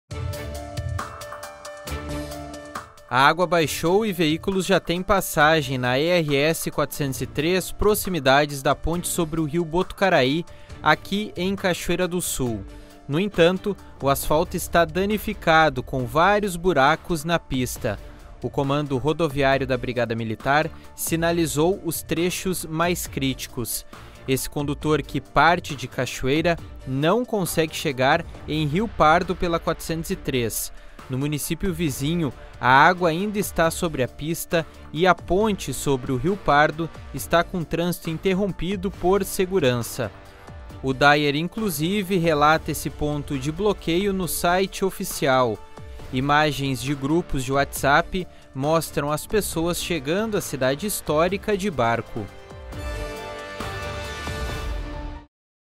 Veículos já trafegam pela ERS-403 em Cachoeira do Sul
Veículos já trafegam pela ERS-403 em Cachoeira do Sul || Em Rio Pardo, a estrada ainda está bloqueada.